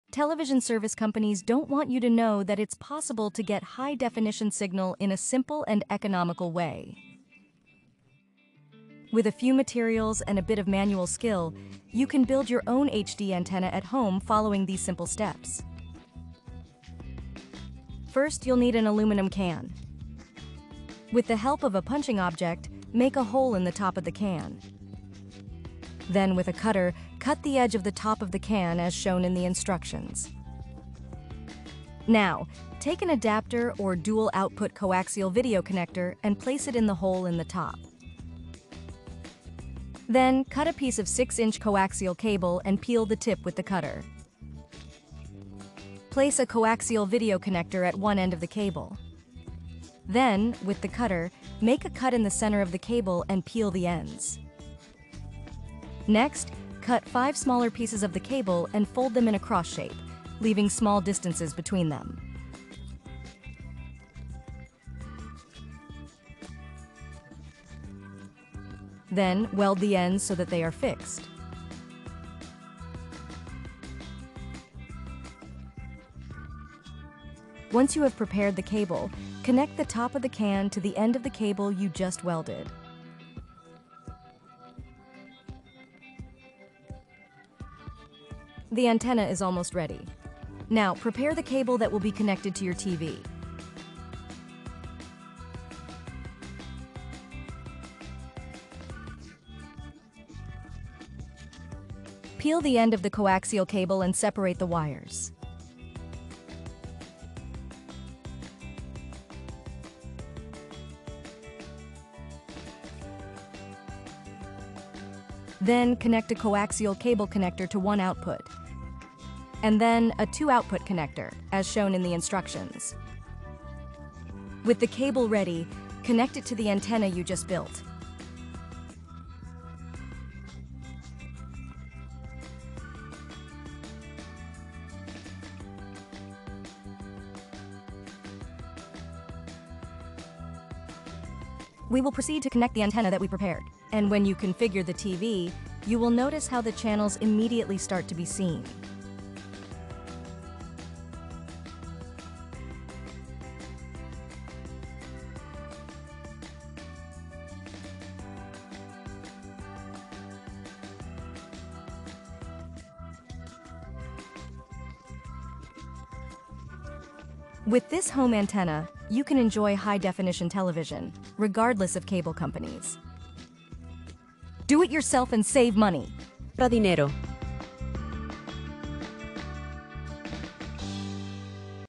The cable companies don't want you to know. Learn how to make an HD antenna quickly and easily!
The cable companies don't want you to know. Learn how to make an HD antenna quickly and easily! Revealed! The Secret to a FREE HD Signal on your TV, Works Instantly!"

Description: Are you tired of paying expensive cable bills for a television signal that could be free? Do not look any further! In this video we will show you how to build a homemade HD antenna with simple materials you have at home. Yes, you read that right, FREE! With just a few easy-to-follow steps, you'll be able to enjoy crystal-clear HD signal on your TV without spending a dime on cable services. Don't miss this opportunity to free yourself from cable television companies and discover how to get the best image quality without paying more. Click and start saving now!